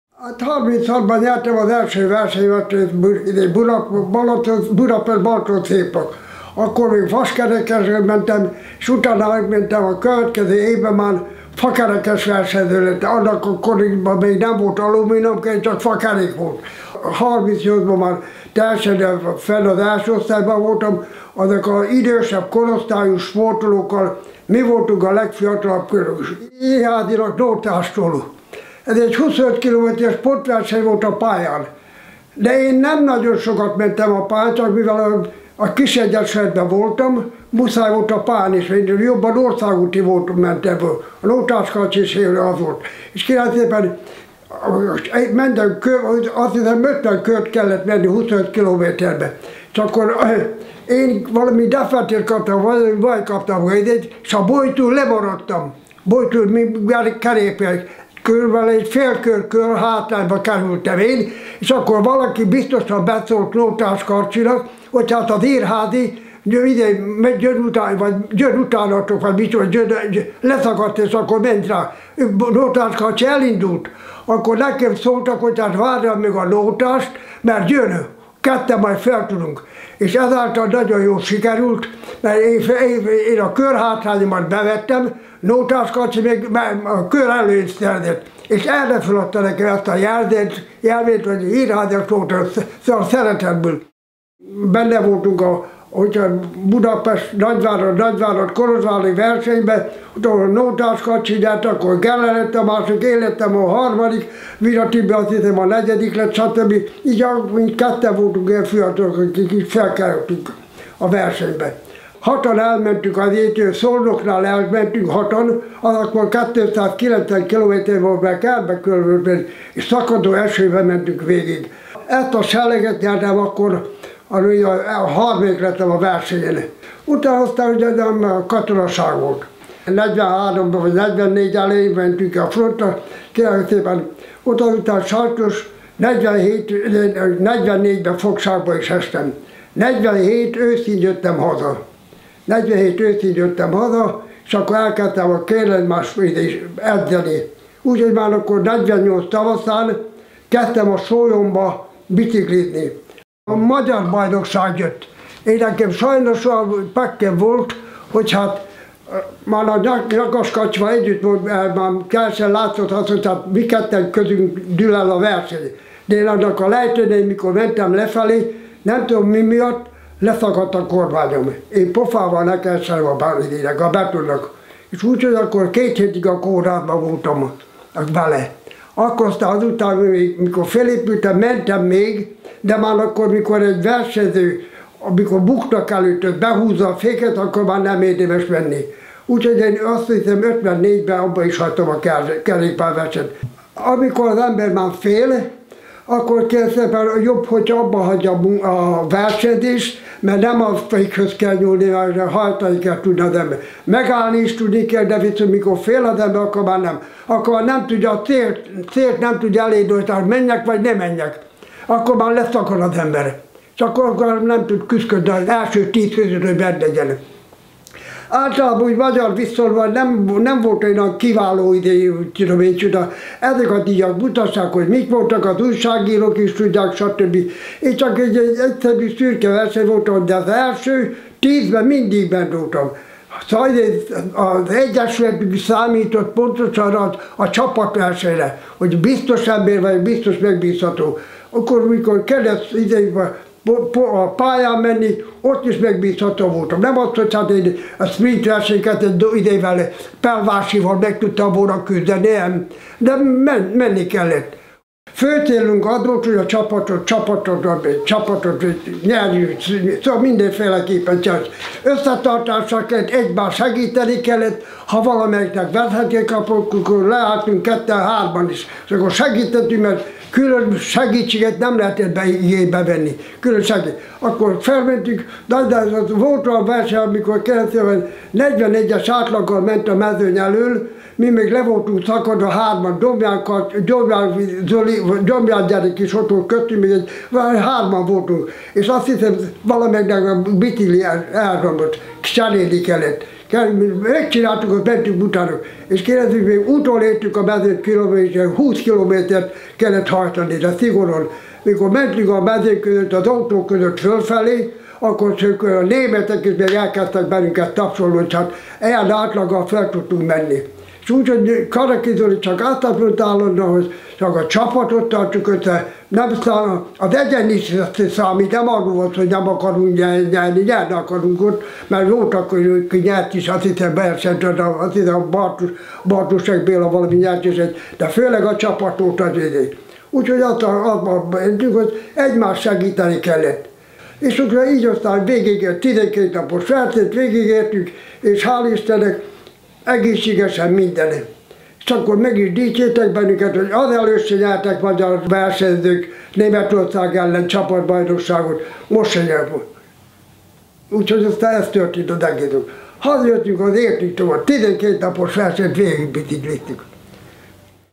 Emlékezés egy Tour-dobogósra
Irházy Mihály, az 1942-es Tour de Hongrie harmadik helyezettje